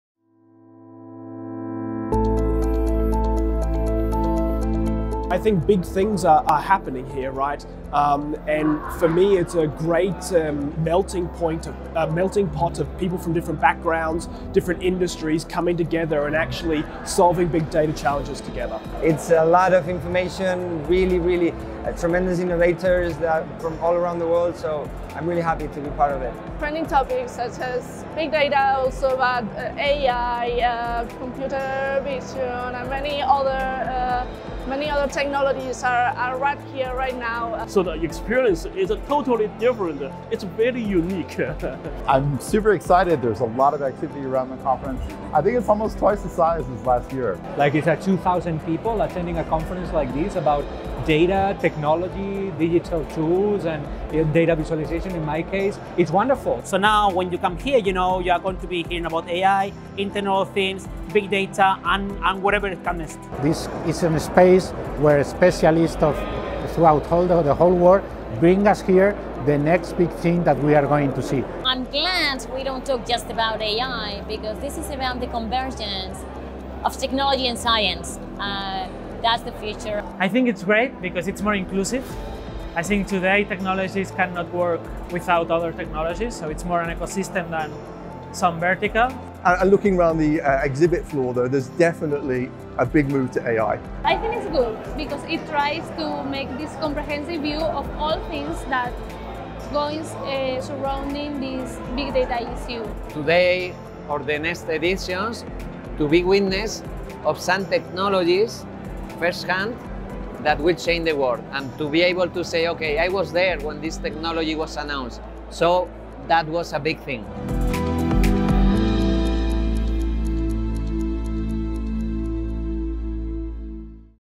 Interview to Big Things 2019 Speakers - What is Big Things Conference for you?
During 7 editions Big Things was known as Big Data Spain. On 2019 the event was rebranded in order to included all the new technologies and challenges that are changing the world. Nowadays Big Things is much more than Big Data. It’s AI, Machine Learning, IoT, Blockchain… It is people from different backgrounds attending a unique conference. Watch this interview to BIGTH19 Speakers and discover what is Big Things Conference for them.

Big Things Conference
20th and 21st November 2019
Kinépolis, Madrid